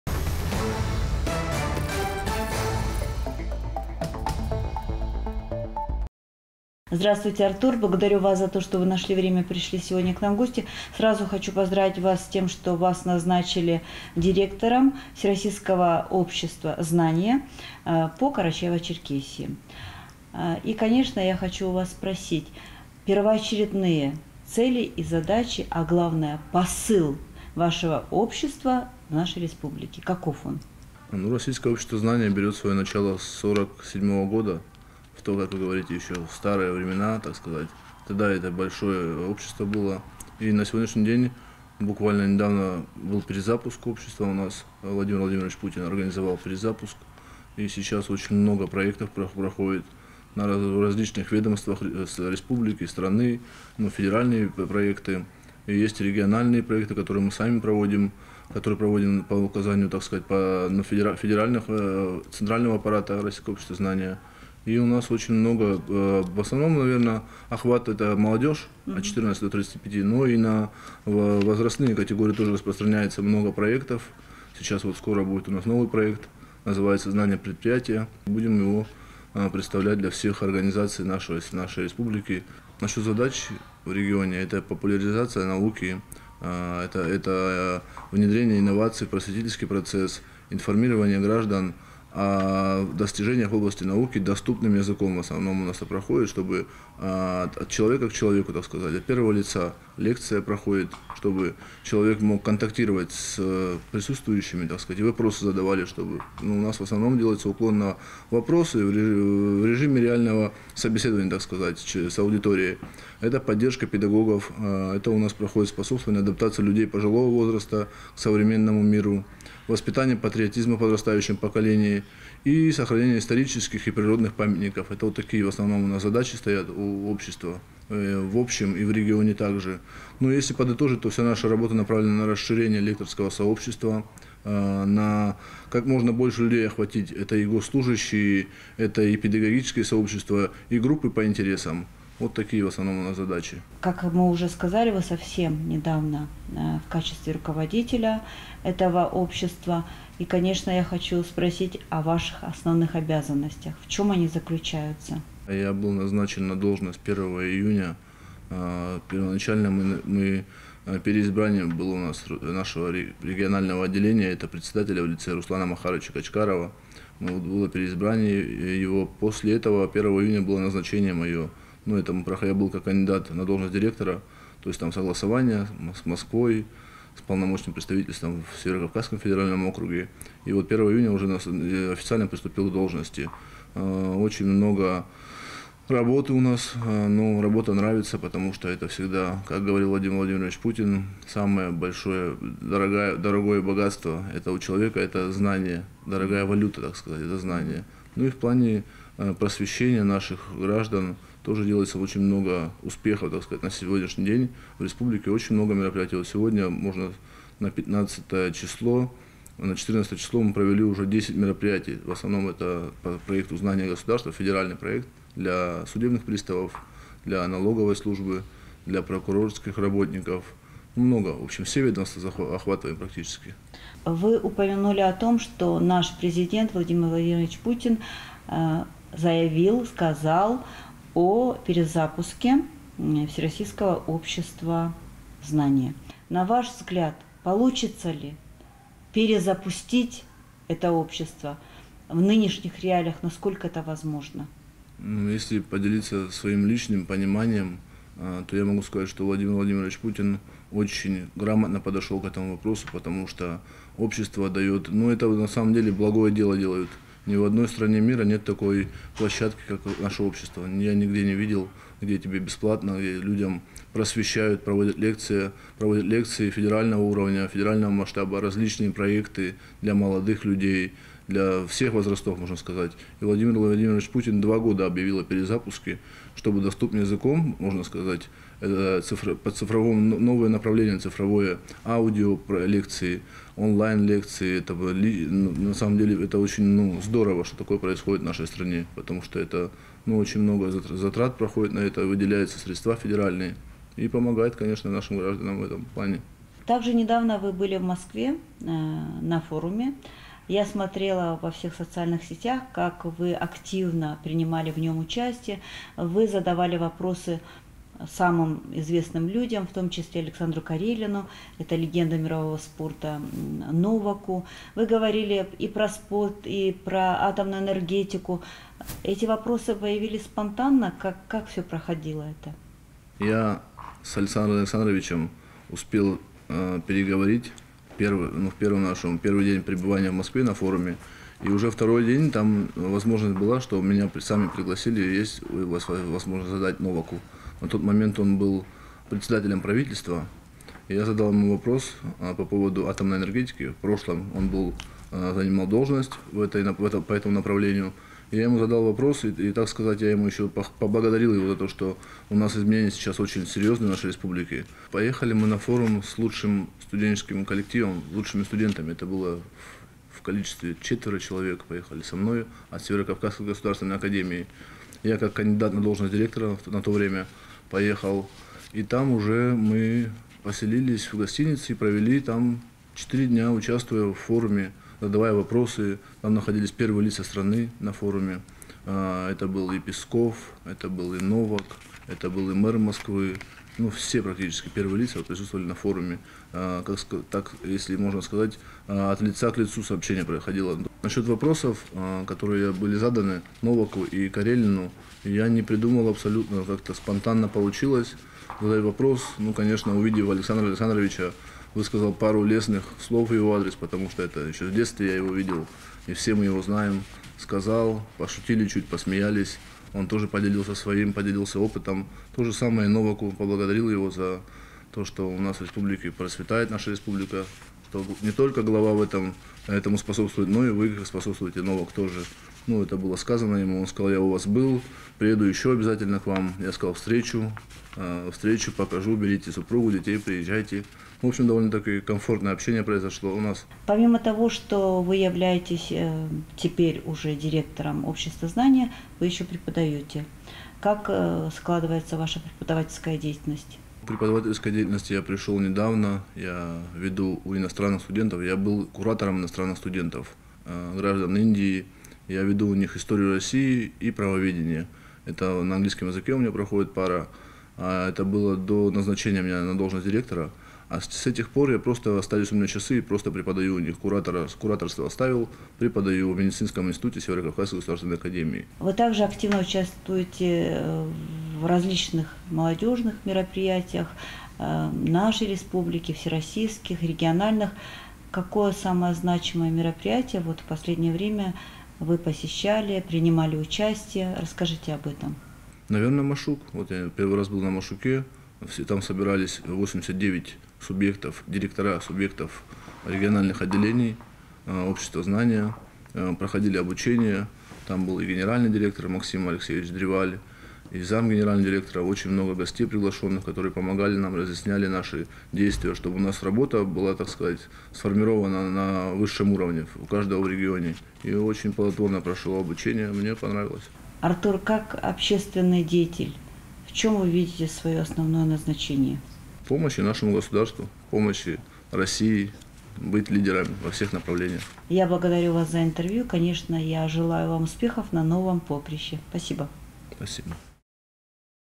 Интервью. Артур Болатов. Эфир от 20.07.2023
Интервью. Артур Болатов, директор филиала "Российское Общество "Знание". Эфир от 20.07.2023